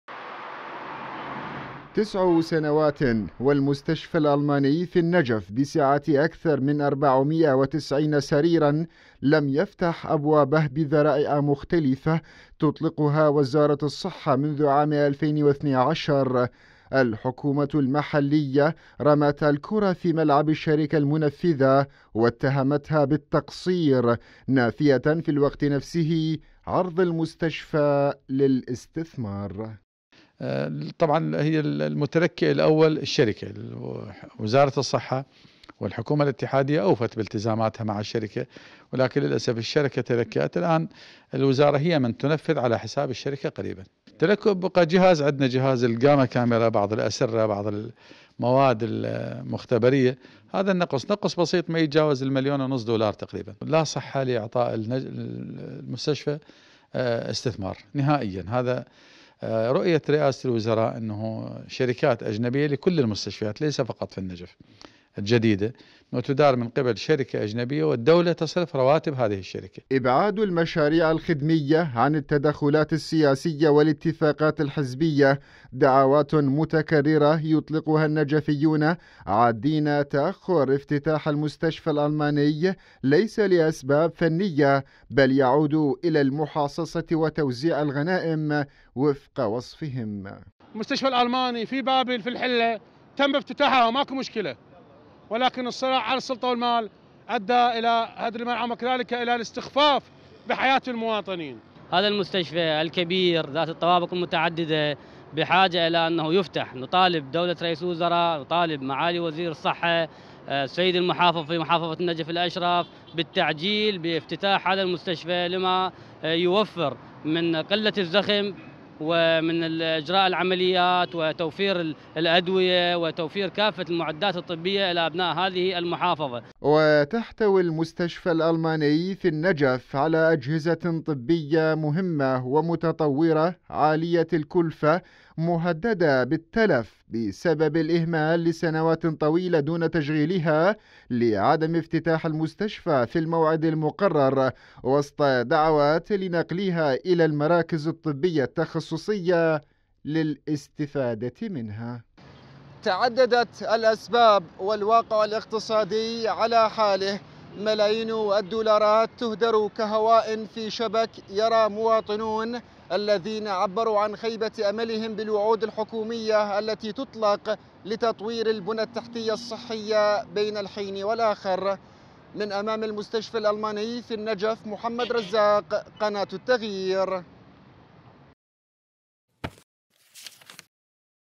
المصالح الحزبية وراء عدم اكمال المستشفى الألماني في النجف | تقرير
دعا ناشطون ومواطنون في النجف الى أبعاد المصالح الحزبية عن المشاريع الخدمية لاسيما المستشفى الألماني في المحافظة الذي تأخر افتتاحه لعدة سنوات، فيما حمل محافظ النجف لؤي الياسري الشركة المنفذة للمستشفى مسؤولية تأخر افتتاحه نافيا في الوقت نفسه عرض المستشفى للاستثمار.